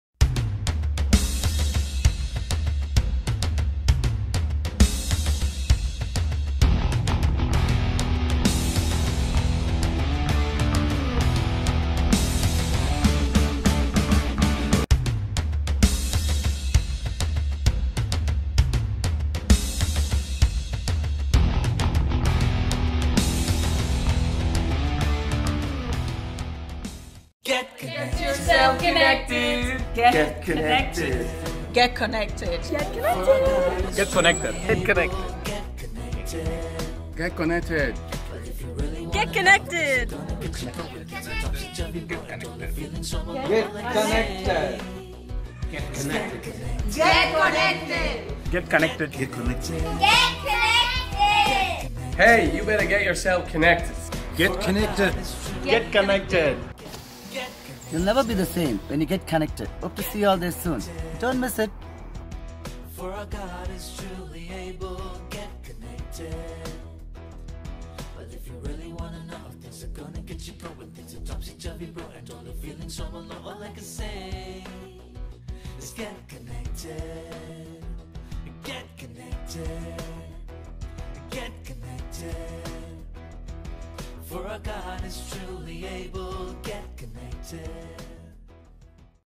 "Get Connected" Kid's Summer Camp 2018 Promo 2 | New Life Church Dublin | July 3 - 7
We welcome you to this exclusive, spectacular & amazing kid's time in this Summer camp with the theme of "GET CONNECTED"
Let's get connected with Love! Peace! Joy!

1, Drumfinn Road,
Ballyfermot,
Dublin 10